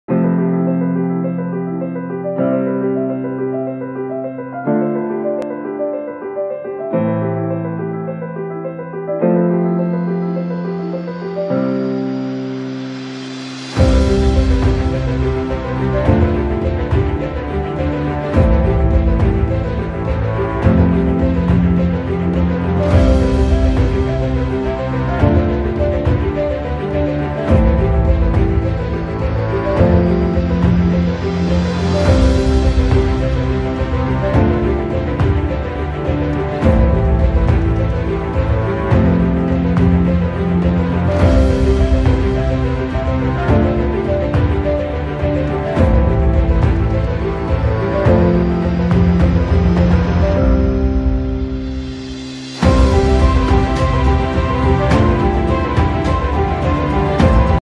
Benefits of Sliding Doors - Schüco Patio Doors | Lite Haus UK
The benefits of a Schüco sliding door installation in your home 🏡